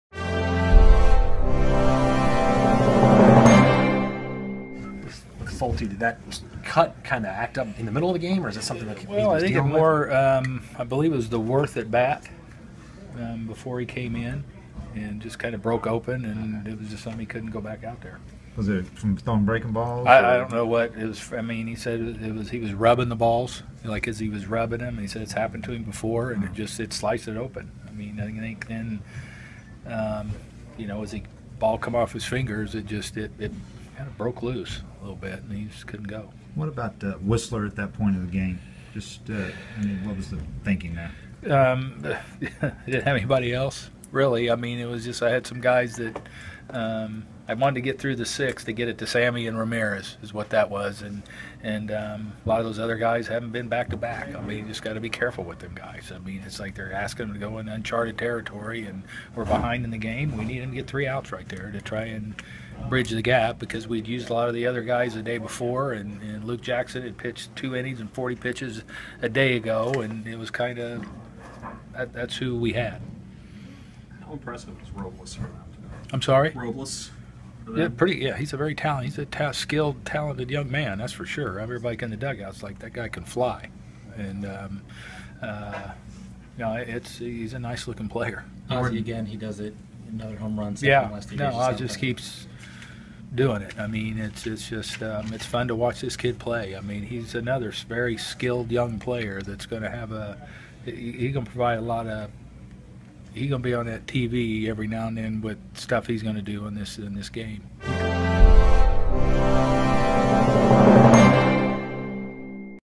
VIDEO: Snitker on Braves' loss and his decison to use Wisler
The Braves had closed to a one-run deficit before Snitker brought in struggling reliever Matt Wisler, who gave up two runs in the sixth inning of a 5-2 loss at Washington. (Video by David O'Brien)